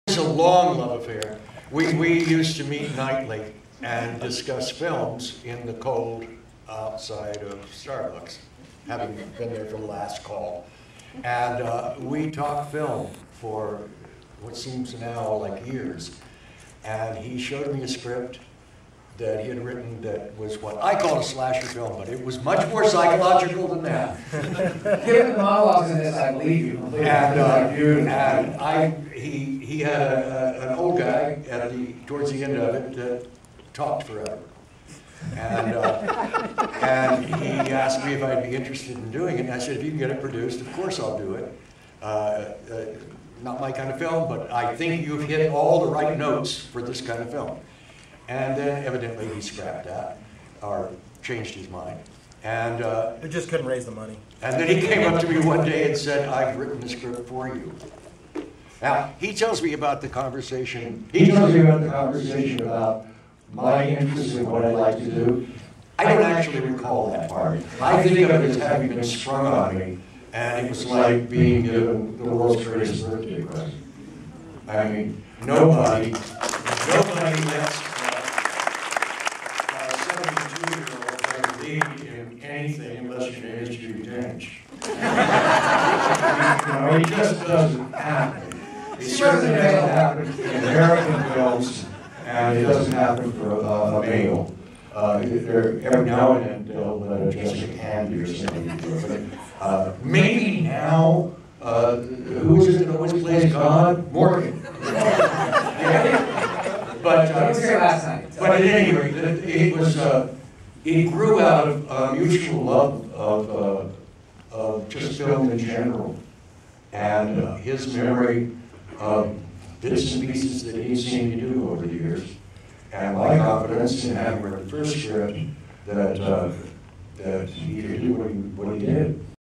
Leon Russom speaking at "The Midnighters" movie at Dances With Films Film Festival 2017
Leon Hussom ( Long time Actor -played Victor) in "The Midnighters" film that show cast at the Dances with Films Film Festival on June 9, 2017 in Hollywood, California.  Video by: Stella Gomez.